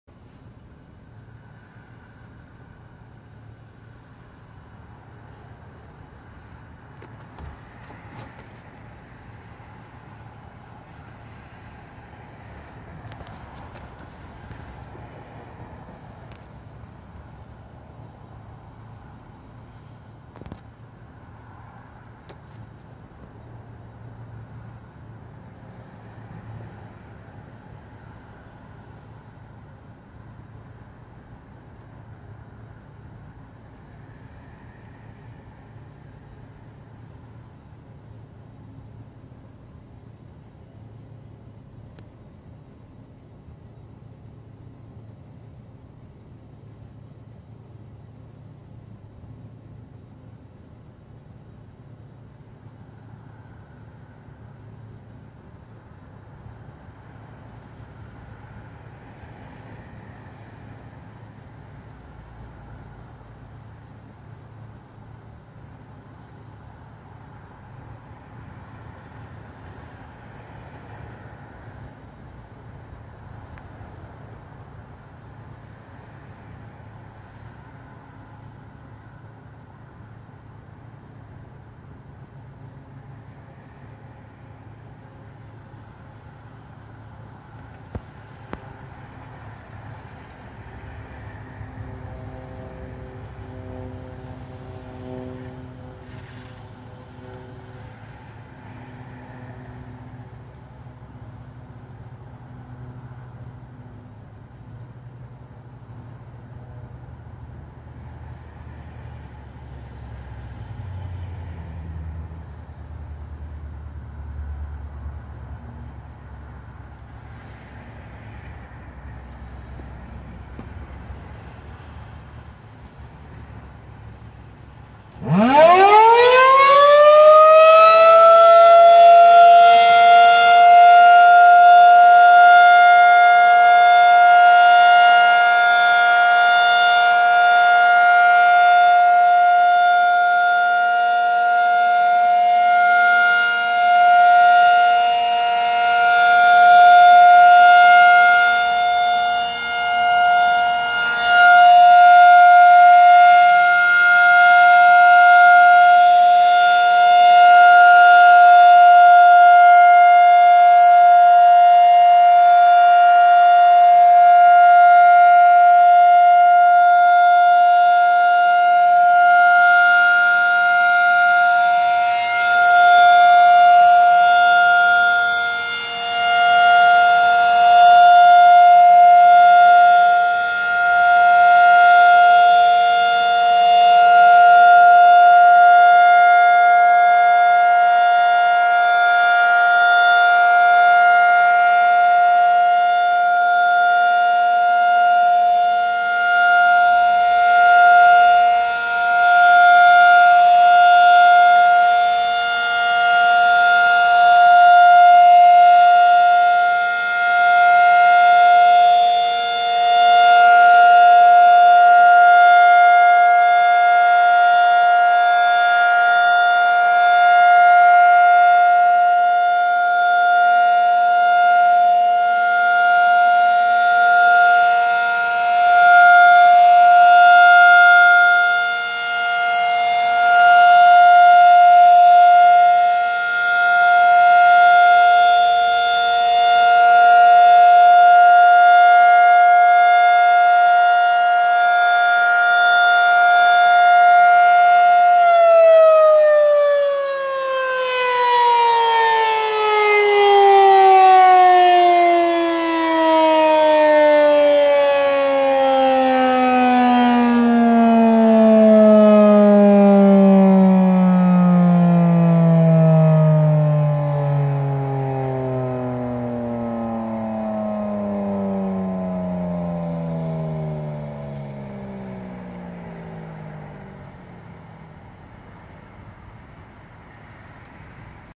Federal Signal 2001 Siren Test 2/5/2022 (Snowblower)
Located in Jackson MI
Test starts every first Saturday of the month at 12:00 PM
Starts at 2:00